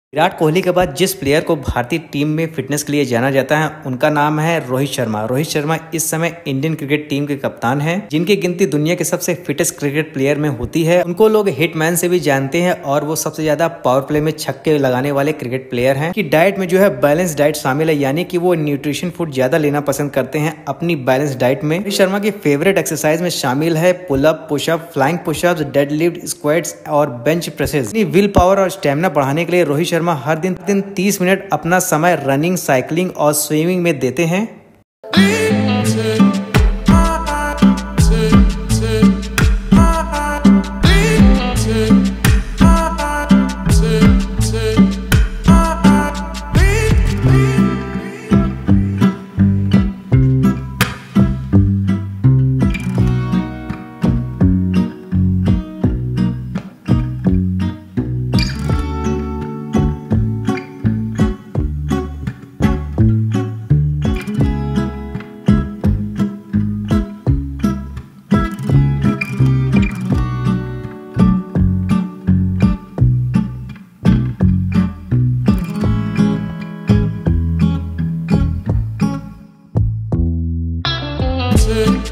Rohit Sharma Fitness Gym Work Out, Daily Food, Flying Push-ups
Rohit Sharma Fitness Gym Work Out, Daily Food, Flying Push-ups. Rohit Sharma is as fit is Virat Kohli. He looks bulky but has Great fitness status.